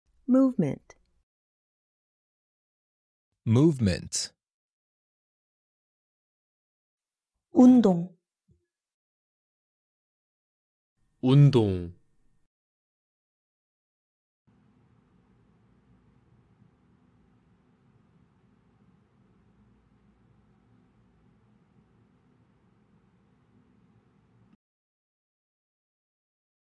영어로 "운동" 말하는 법
남자 목소리 :
Movement

여성 목소리 :
Movement

남성 음성 번역 :
운동

여성 음성 번역 :
운동